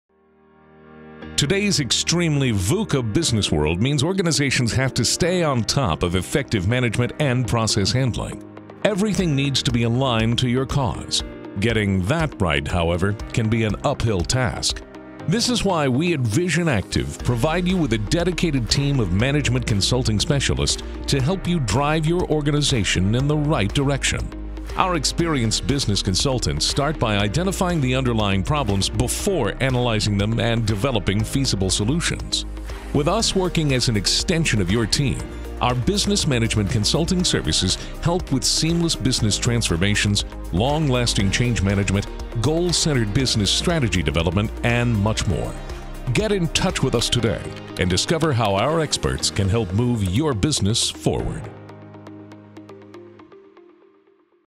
A Business Consulting Firm that can Help in Organisation’s Performance Management
Our experienced and dedicated business consultants can help your company with:
• Strategy Development
• Performance Management
• Business Transformation
• Change Management
• Organisational Diagnostic analysis
• Organisational Turnaround

Today’s extremely ‘VUCA’ business world means organizations have to stay on top of effective management and process handling. Everything needs to be aligned to your cause. Getting that right, however, can be an uphill task.

This is why we at Vision Activ provide you with a dedicated team of management consulting specialists to help you drive your organization in the right direction. Our experienced business consultants start by identifying the underlying problems before analyzing them and developing feasible solutions.

With us working as an extension of your team, our business management consulting services help with seamless business transformations, long-lasting change management, goal-centered business strategy development, and much more. Get in touch with us today and discover how our experts can help move your business forward.

Talk with our experts today and discover how we can help you transform your organization.